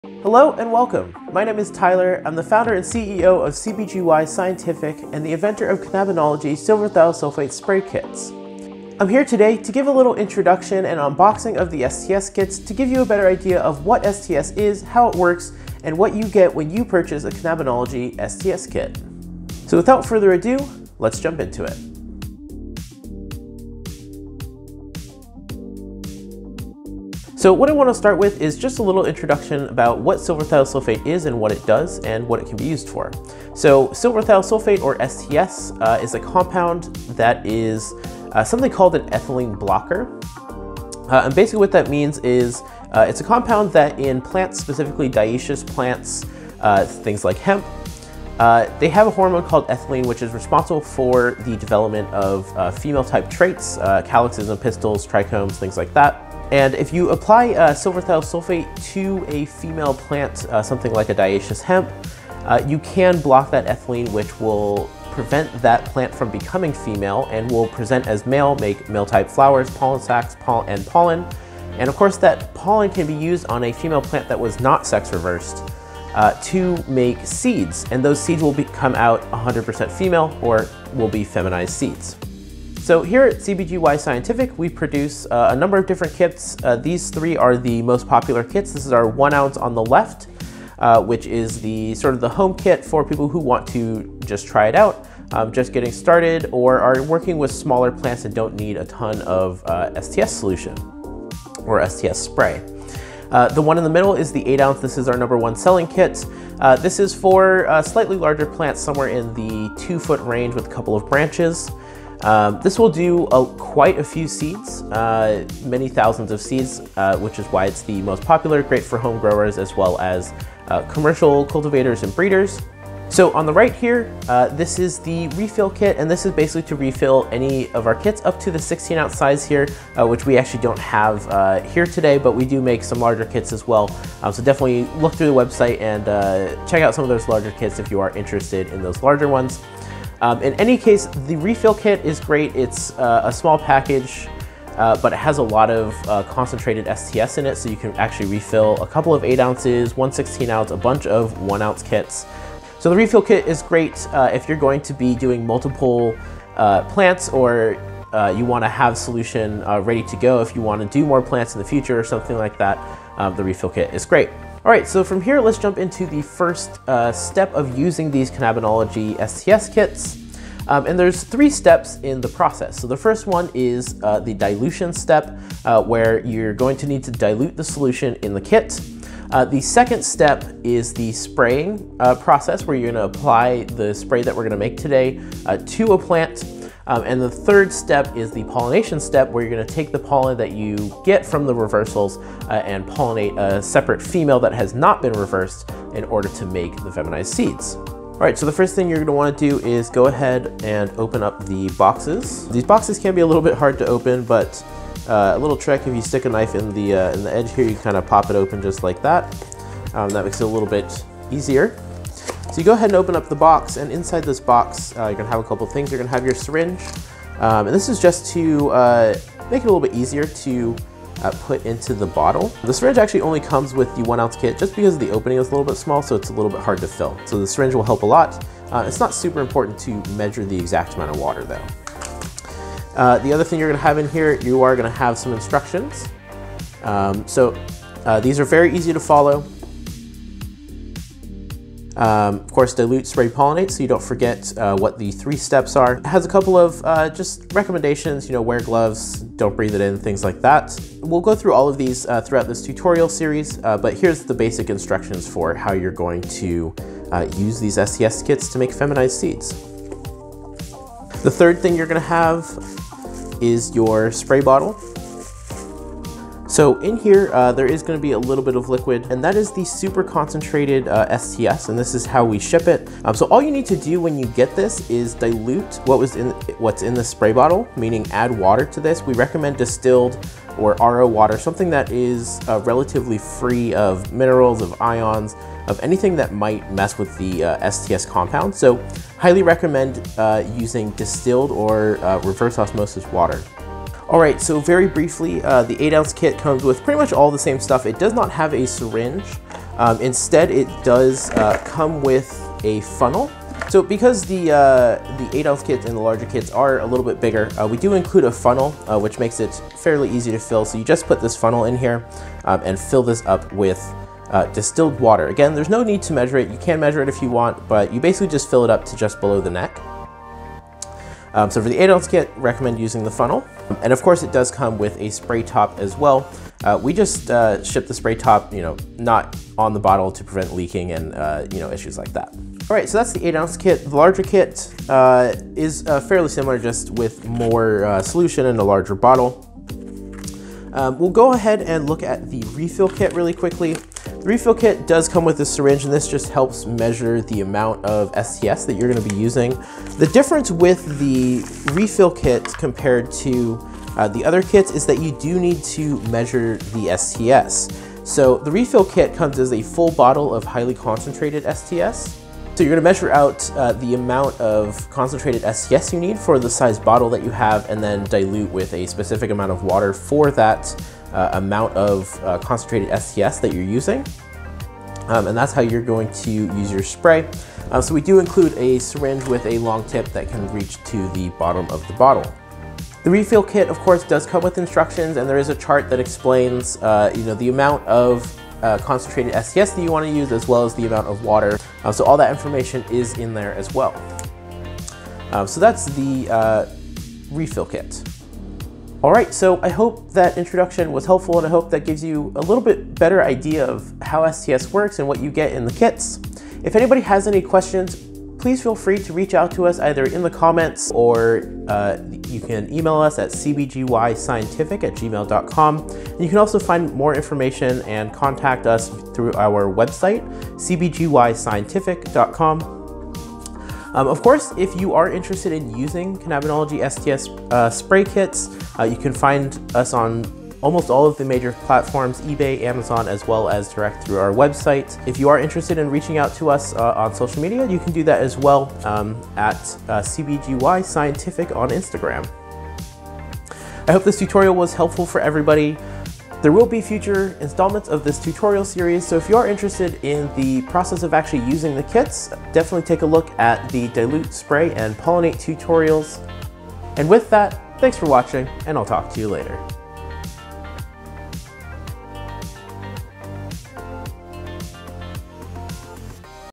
Introduction and Unboxing: Cannabinology Silver Thiosulfate Spray Kits
Introducing Cannabinology Silver Thiosulfate Spray Kits produced by us here at CBGY Scientific. These kits are a simple and effective way to make feminized seeds at home or in a commercial setting.